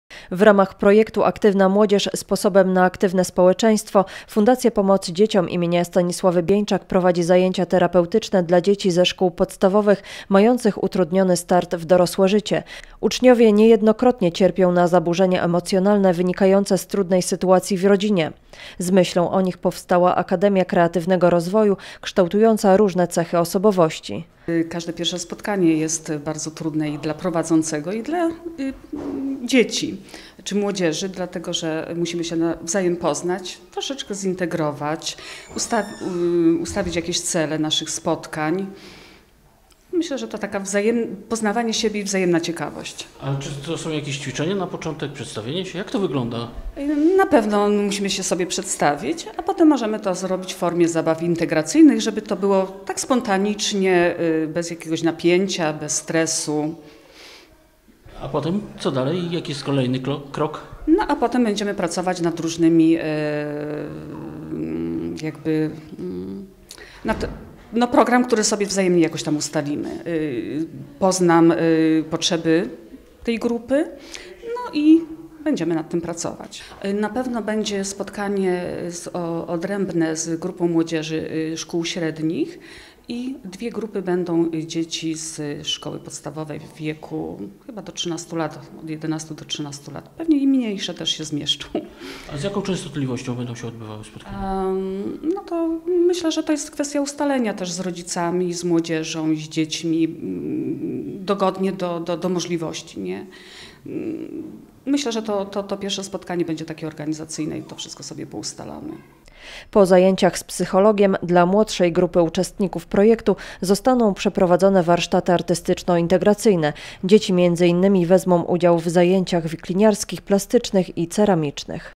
Akademia Kreatywnego Rozwoju
W ramach projektu "Aktywna młodzież sposobem na aktywne społeczeństwo" Fundacja Pomocy Dzieciom im. Stanisławy Bieńczak prowadzi zajęcia terapeutyczne dla dzieci ze szkół podstawowych, mających utrudniony start w dorosłe życie. Uczniowie niejednokrotnie cierpią na zaburzenia emocjonalne wynikające z trudnej sytuacji w rodzinie. Z myślą o nich powstała Akademia Kreatywnego Rozwoju kształtująca różne cechy osobowości.